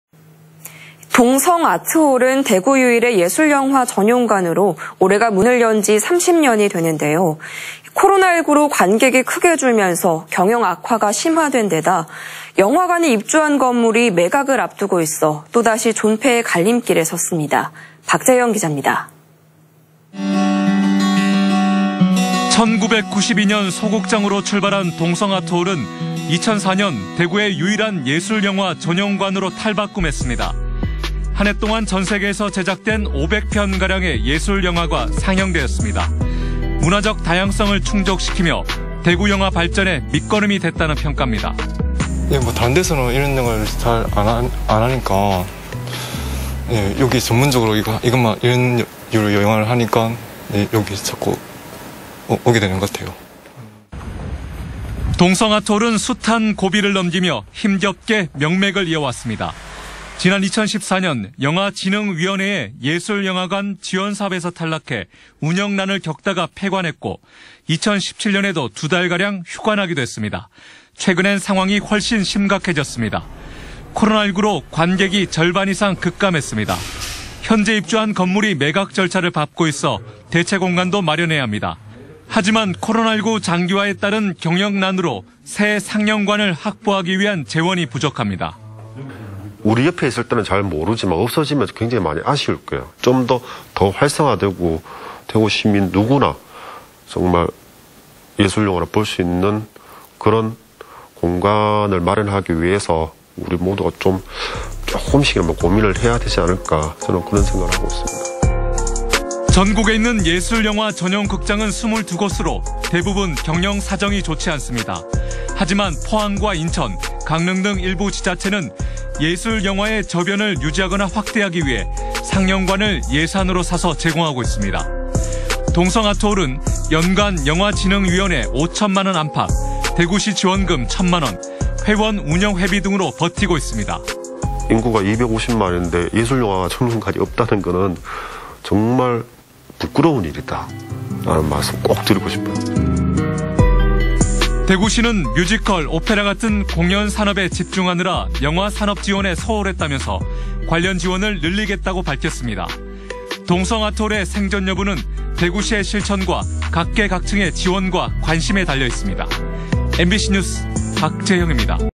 동성아트홀'존폐 갈림길..지원책 마련해야/ 안동MBC
.2021/10/04 15:41:35    작성자 : 박재형

◀ＡＮＣ▶
'동성 아트홀'은
대구 유일의 예술영화 전용관으로
올해가 문을 연 지 30년이 되는데요.

코로나 19로 관객이 크게 줄면서
경영 악화가 심화된데다,
영화관이 입주한 건물이 매각을 앞두고 있어
또 다시 존폐의 갈림길에 섰습니다.

박재형 기자
◀ＥＮＤ▶
◀ＶＣＲ▶
1992년 소극장으로 출발한 동성 아트홀은
2004년 대구의 유일한 예술영화 전용관으로
탈바꿈했습니다.

한 해 동안 전 세계에서 제작된 500편가량의
예술영화가 상영돼 왔습니다.

문화적 다양성을 충족시키며
대구 영화 발전의 밑거름이 됐다는 평가입니다.

◀ＩＮＴ▶이승배/직장인
"다른 데에서는 이런 영화를 잘 안 하니까 여기는 전문적으로 이런 영화를 하니까 여기 자주 오게 되는 것 같아요."

동성아트홀은 숱한 고비를 넘기며
힘겹게 명맥을 이어 왔습니다.

지난 2014년 영화진흥위원회의 예술영화관
지원사업에서 탈락해 운영난을 겪다가 폐관했고
2017년에도 두 달가량 휴관하기도 했습니다.

최근에는 상황이 훨씬 심각해졌습니다.

코로나 19로 관객이 절반 이상 급감했습니다.

현재 입주한 건물이 매각절차를 밟고 있어
대체 공간도 마련해야합니다.

하지만 코로나19 장기화에 따른 경영난으로
새 상영관을 확보하기 위한 재원이 부족합니다.

◀ＩＮＴ▶윤성근 프로그래머/대구 동성아트홀
"우리 옆에 있을 때는 모르지만, 없어지면 굉장히 아쉬울 거예요. 좀 더 활성화되고 대구시민 누구나 예술 영화를 볼 수 있는 그런 공간을 마련하기 위해 조금씩 고민을 해야 하지 않겠나 생각합니다."

전국에 있는 예술영화 전용 극장은 22곳으로
대부분 경영 사정이 좋지 않습니다.

하지만 포항, 인천, 강릉 등 일부 지자체는
예술 영화의 저변을 유지하거나 확대하기 위해
상영관을 예산으로 사서 제공하고 있습니다.

동성 아트홀은
연간 영화진흥위원회 5천만 원 안팎,
대구시 지원금 천만 원, 회원 운영 회비 등으로
버티고 있습니다.

"인구 250만인데 예술 전용관이 없다는 것은 정말 부끄러운 일이다. 이 말을 꼭 드리고 싶어요."

대구시는
뮤지컬, 오페라 같은 공연산업에 집중하느라
영화 산업 지원에 소홀했다면서
관련 지원을 늘리겠다고 밝혔습니다.

동성아트홀의 생존 여부는 대구시의 실천과
각계 각층의 지원과 관심에 달려 있습니다.

MBC 뉴스 박재형입니다.